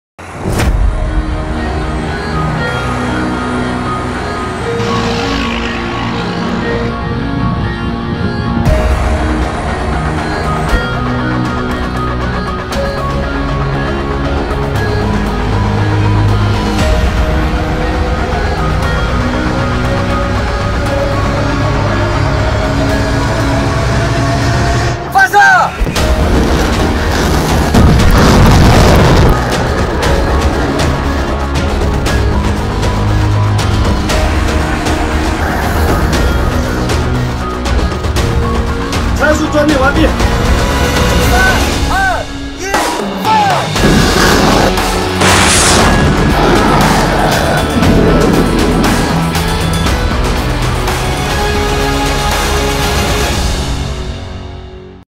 中国陆军远程火箭炮部队大漠演兵
近日，中国陆军第77集团军某炮兵旅在西北某综合训练场组织了一场实弹射击演练。全面检验了远火武器系统作战所需的指挥控制、火力打击、战场机动、作战保障等多种能力。